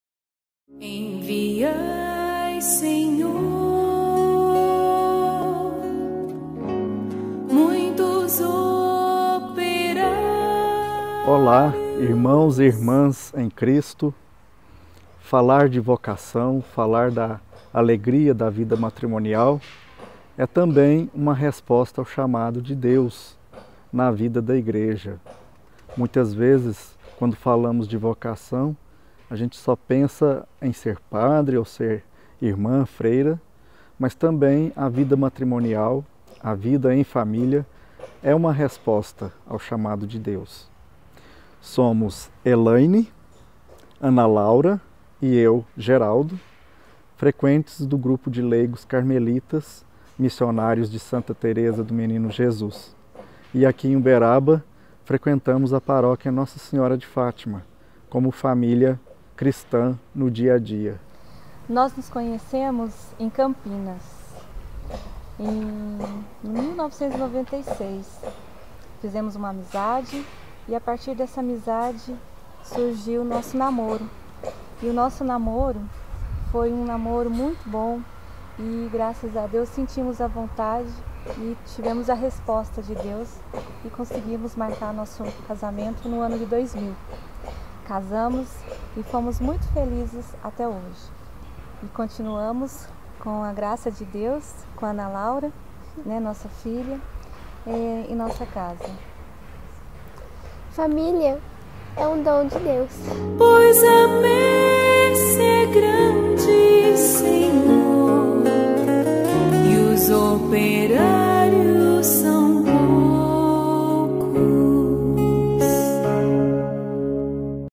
Testemunho Vocacional: Família Gonçalves de Lima | Vocação Familiar
A Arquidiocese de Uberaba, atenta ao pedido de Jesus: “Pedi ao Senhor da messe que envie operários para a sua messe” (Mt 9,38), iniciou em maio a Ação Evangelizadora: cada comunidade uma nova vocação, em prol de vocações para a Igreja.

No vídeo de hoje, a família Gonçalves de Lima apresenta o seu testemunho vocacional.

Nos ajude a divulgar amplamente esta Ação Evangelizadora!
Rezemos pelas vocações!